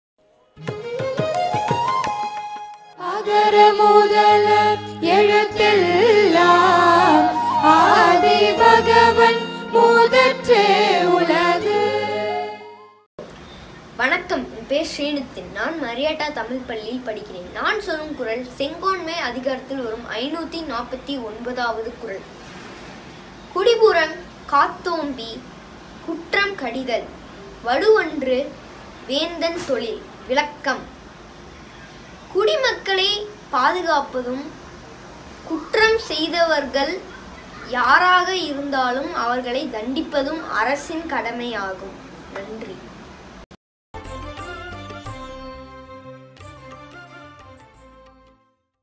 336 ஸ்ரீநிதின் வேணுகோபால் திருக்குறள் நேரம் குறள் எண் 549
அதிகாரம்: செங்கோன்மை

குடிபுறங் காத்தோம்பிக் குற்றம் கடிதல்
வடுவன்று வேந்தன் தொழில்